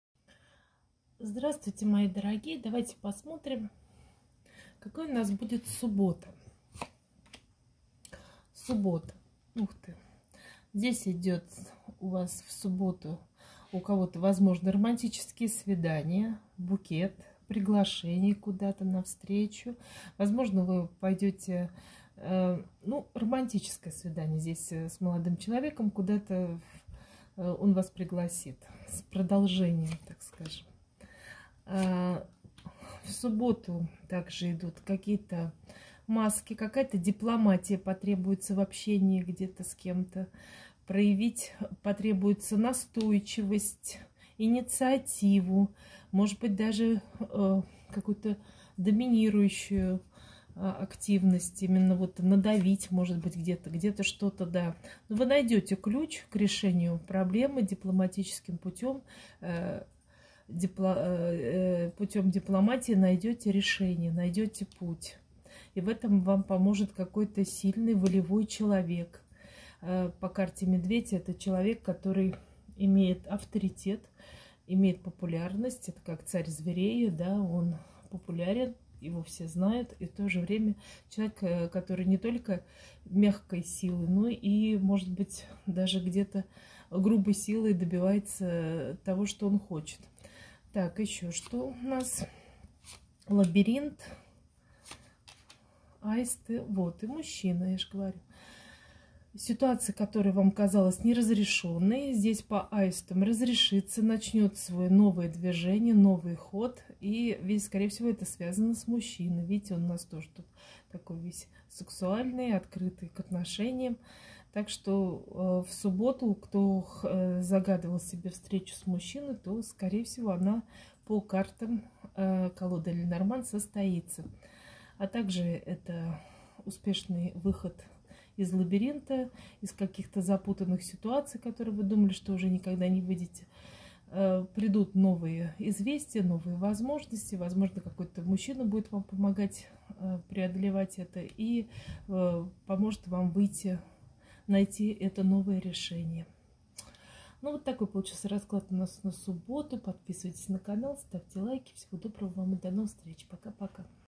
Прогноз на субботу 29 октября 2022 г.
Личные расклады не делаю.
ОБЩИЙ расклад карт ТАРО. Все прогнозы общие и носят развлекательный характер и могут не совпадать с вашей реальностью.